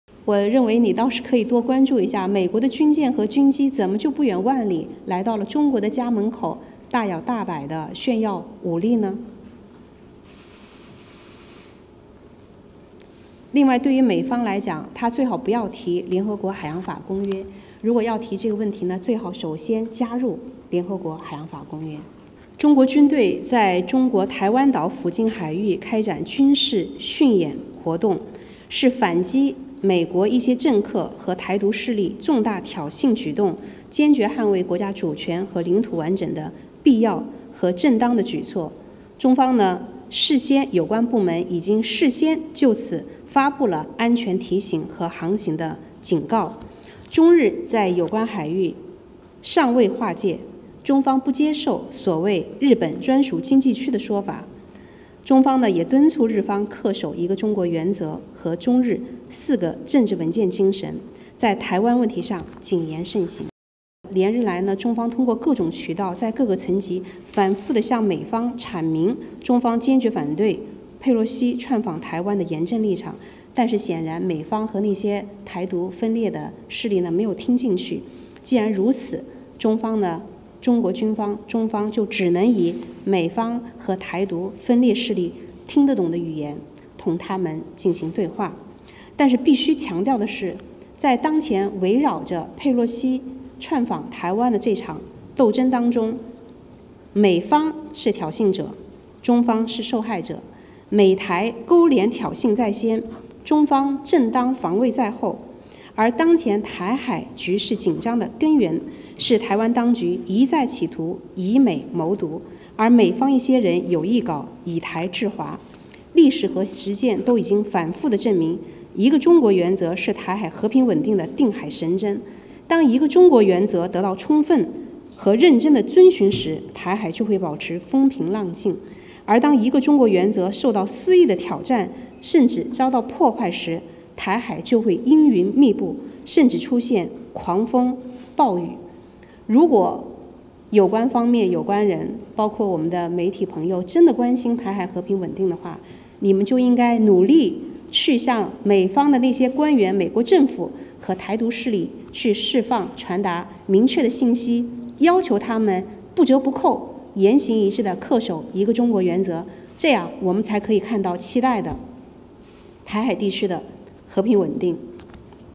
China PLA drills 'speak' to US, 'Taiwan independence' separatists in a way that they can understand
Hua Chunying: On the actions taken by the Chinese military, I’m sure you have seen the statements made by the spokespersons of the Ministry of National Defense and the PLA Eastern Theater Command. The actions are aimed at deterring the “Taiwan independence” forces, so they are legitimate and necessary.

We have never seen any problems with the freedom of navigation in relevant waters. I think what truly needs our attention is how the US military vessels and aircraft would fly thousands of miles all the way from their homeland just to flex muscles at the doorstep of China. And also for the US side, maybe it is better not to mention UNCLOS. If the US want to mention it, please join UNCLOS now first. Taiwan, being a part of China, has no right to claim “internal waters”.

Hua Chunying: The Chinese military has carried out exercises and training in the waters off China’s Taiwan island. This is necessary and legitimate as a response to the major provocation of the US side and the “Taiwan independence” forces and as a step to firmly uphold China’s sovereignty and territorial integrity. The relevant Chinese authorities have issued safety alerts and navigation warnings. Since China and Japan have not yet carried out maritime delimitation in relevant waters, China does not accept the notion of so-called “Japanese EEZ”. The Chinese side urges Japan to uphold the one-China principle and the spirit of the four political documents between the two countries and be prudent on the Taiwan question.

Some of you just mentioned the actions taken by China’s military, and we’ve also noted the remarks of the US side on this issue. I’d like to say a bit more about this.

During the past few days, through multiple channels and at various levels, we have repeatedly made clear our firm opposition to Pelosi’s visit to the US side, but apparently the US side and the “Taiwan independence” separatist forces have not taken the message to heart. In that case, the Chinese side is left with one option only, which is to “speak” to them in a way that they can understand.

The important thing is, while expressing our serious opposition to Pelosi’s visit, we need to be clear that it is the US who is the provocateur, and China the victim. The US side colluded with Taiwan and made provocations first, whereas China has been compelled to act in self-defense. The root cause of the tensions in the Taiwan Strait is that the Taiwan authorities have been soliciting US support to seek “Taiwan independence”, and certain individuals in the US intend to use the Taiwan question to contain China. History and reality have proved time and again that the one-China principle is what underpins stability across the Taiwan Strait. When the one-China principle is fully recognized and earnestly followed, the Taiwan Strait would remain calm and tranquil. However, when the one-China principle is willfully challenged or even sabotaged, there would be dark clouds or even violent storms across the Taiwan Strait.

If people truly care about peace and stability in the Taiwan Strait, particularly friends from the press, it is very important for you to send a clear message to the US officials and to “Taiwan independence” forces about the need to strictly abide by the one-China principle in both words and deeds. That is how we can truly see peace and stability in the Taiwan Strait.